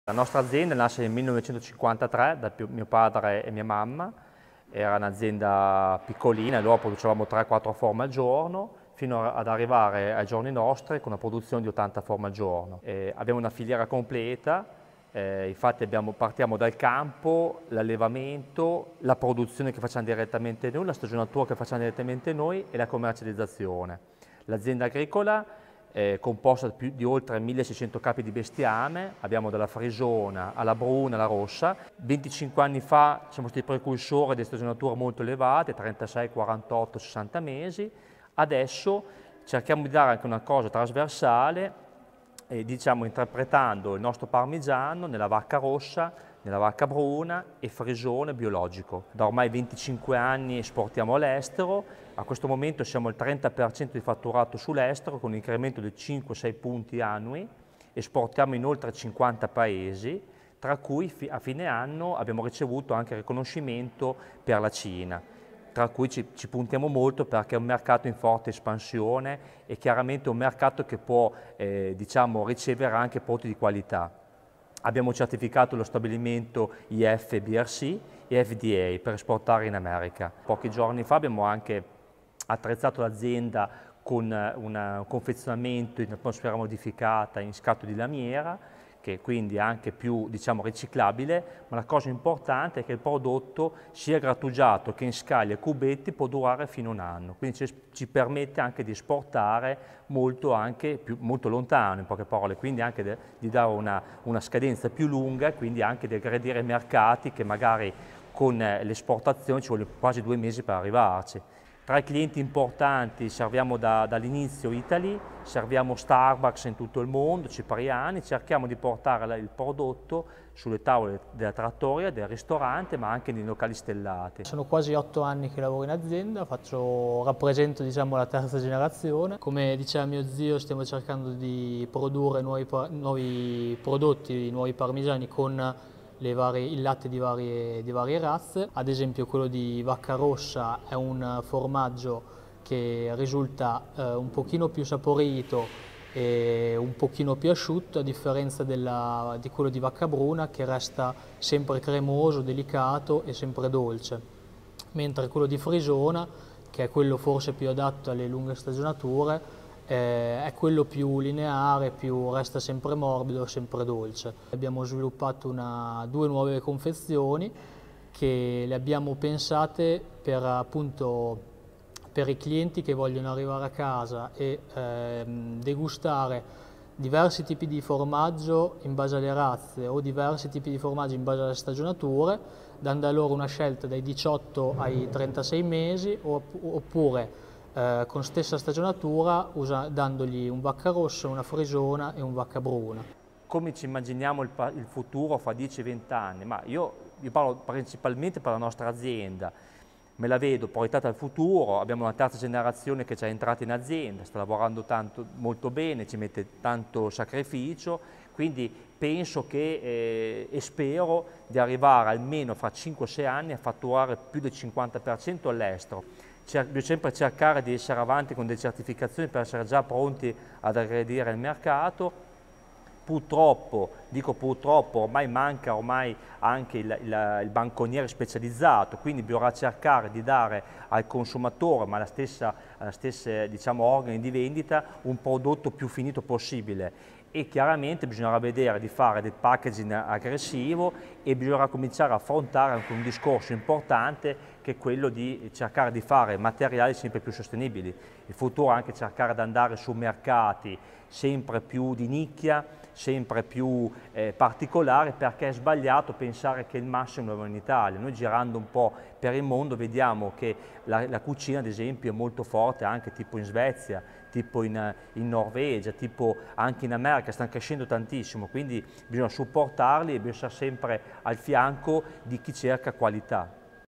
Paolo e Andrea Gennari sui programmi presenti e futuri del loro Caseificio
Paolo Gennari, titolare del Caseificio Gennari s.r.l. di Collecchio, assieme al nipote Andrea, ci descrivono quali sono i loro progetti di sviluppo imprenditoriali, in un'azienda in cui alla tradizionale attenzione al prodotto viene affiancata una naturale apertura al mercato e alle innovazioni.